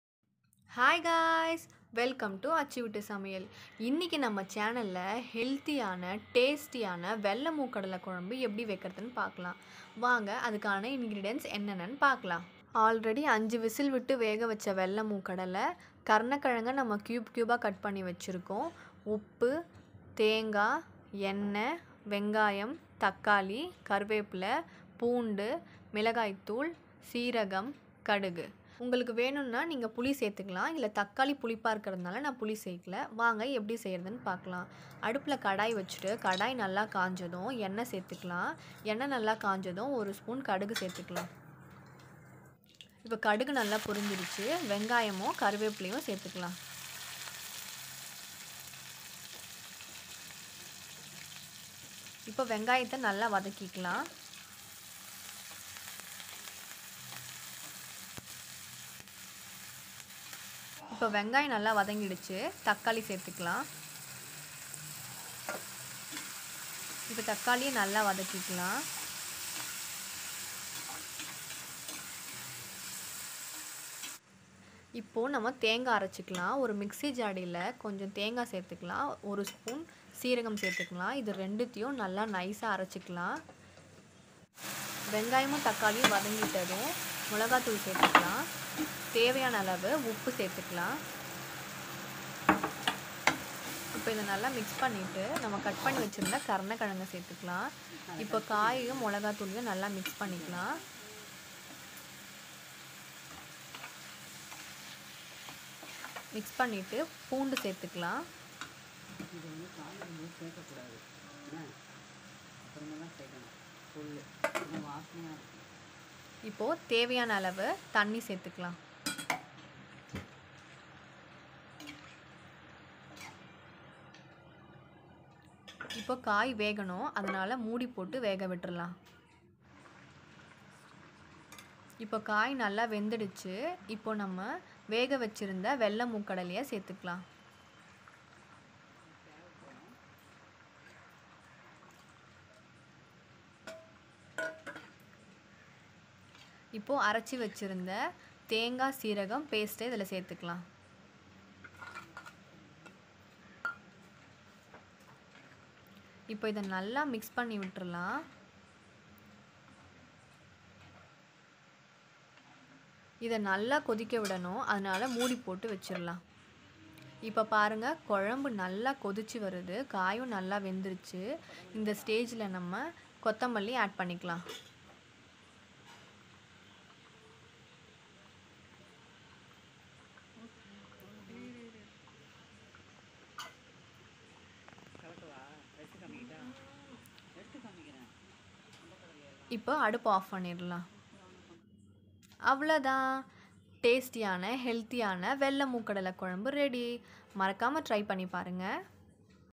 healthy vellai mookadalai kozhambu/white chenna curry/வெள்ளை மூக்கடலை கொழும்பு@achu veetu samayal
healthy and tasty vellai mookadalai kozhambu in different way.

try this and comment.